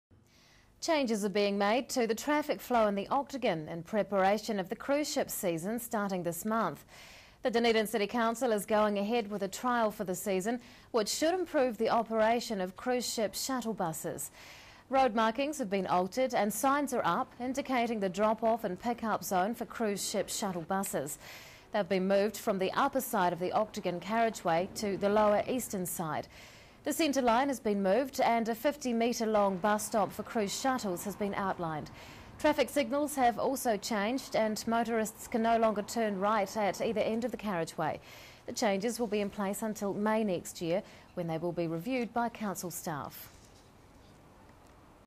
Octagon traffic flow altered for cruise ship season
Changes are being made to the traffic flow in the Octagon, in preparation of the cruise ship season starting this month.